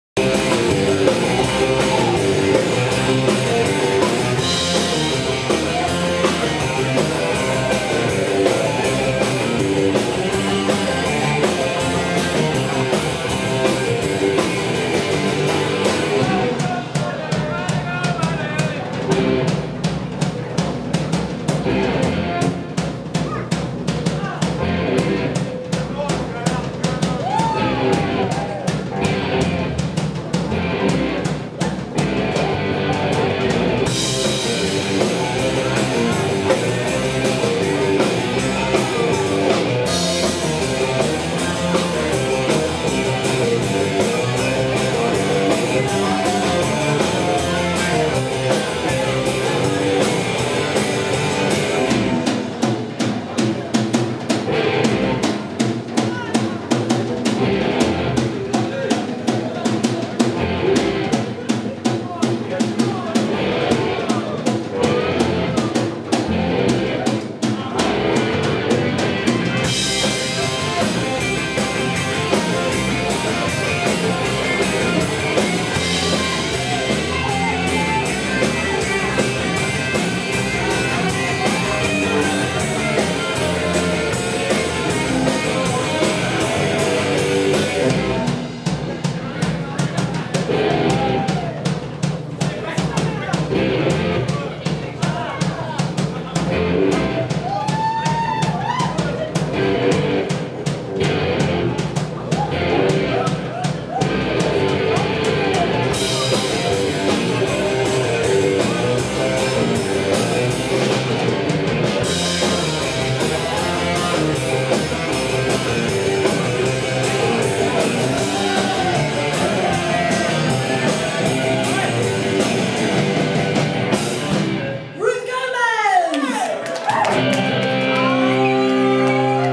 joan ov arc 08.MTS
Viral=video presents UK all girl post-modern rock band Joan Ov Arc playing at the Purple Emperor Harlow May 2011, with Ruth guesting on the pots 'n pans.